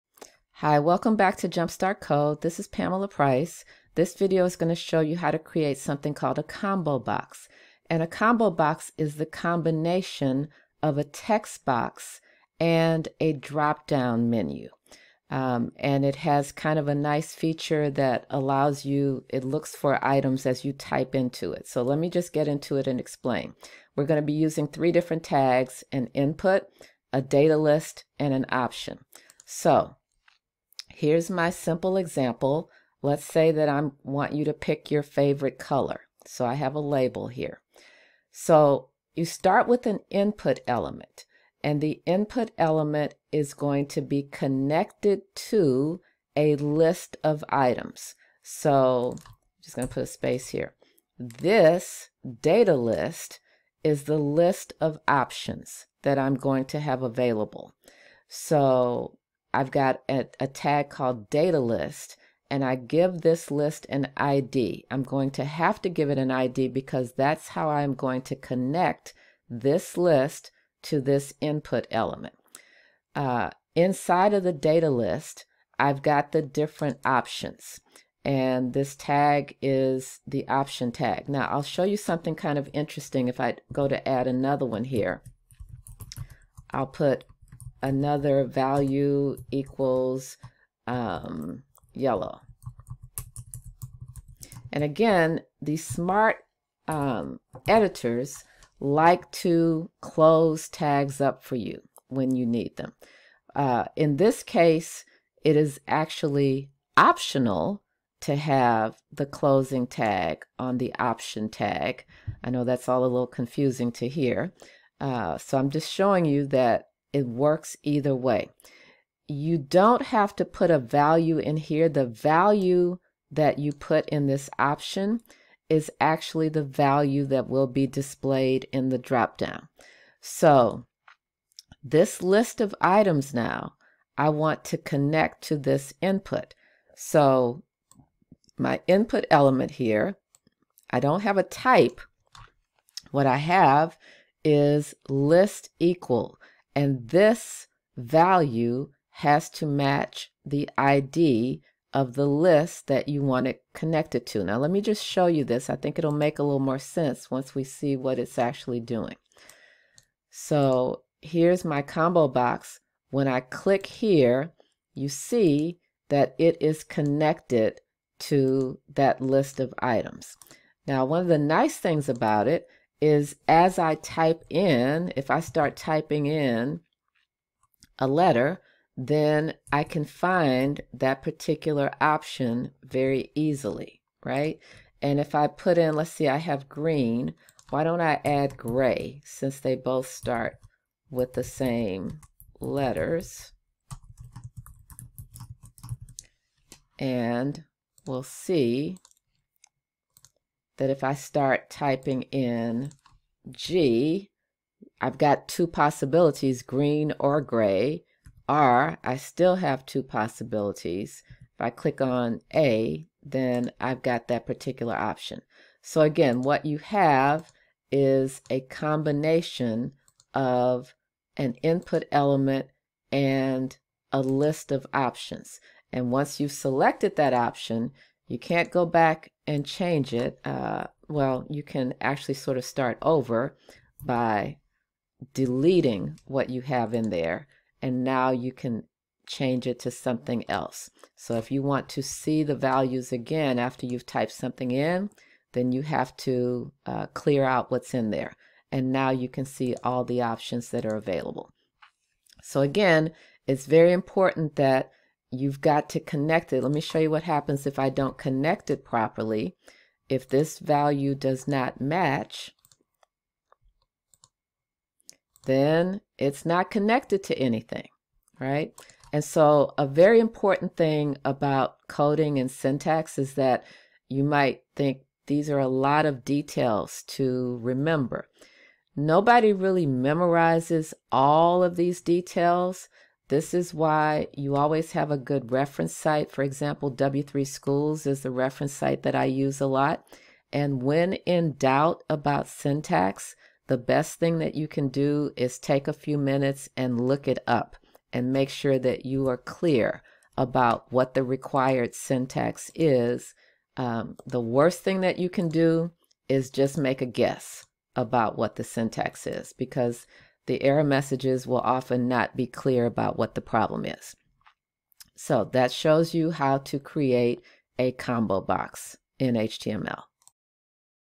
Using datalist to create a combo box in HTML
Using input, datalist, and option tags